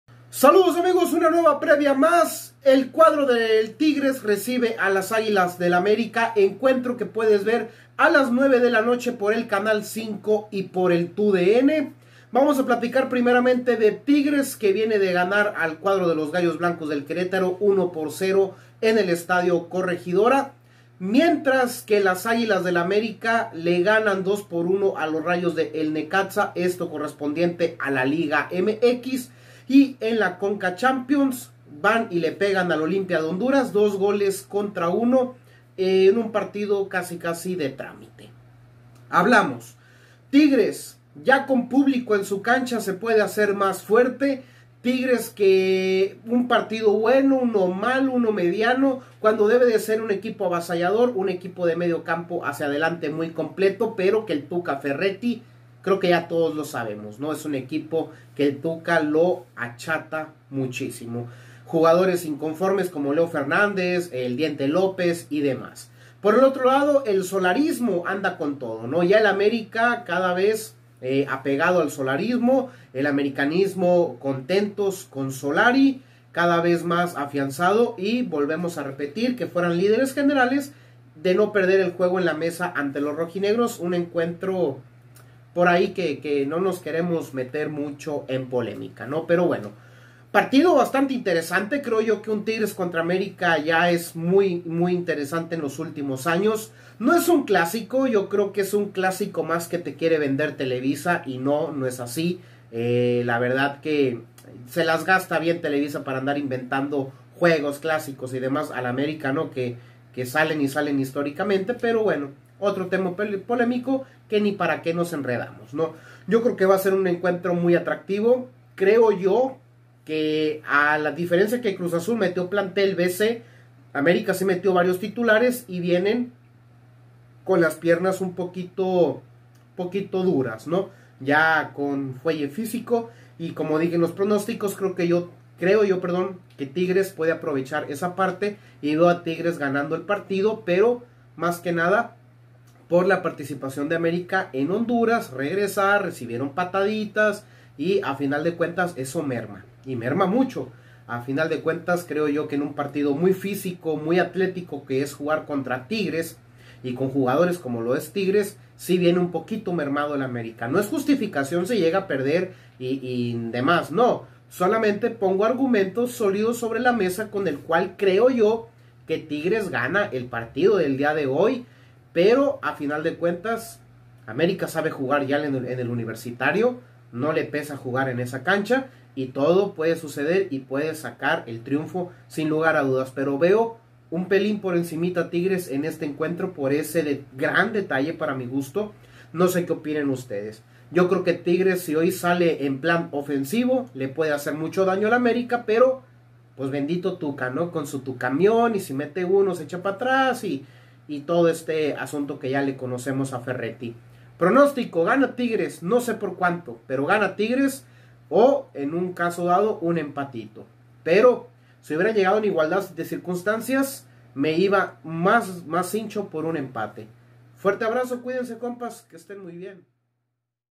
Tigres vs América Jornada 14 Liga MX Guardianes 2021.
Los tigres de la uanl reciben a las aguilas de el América en partido correspondiente a la jornada 14 del torneo guardianes 2021 de la liga mx.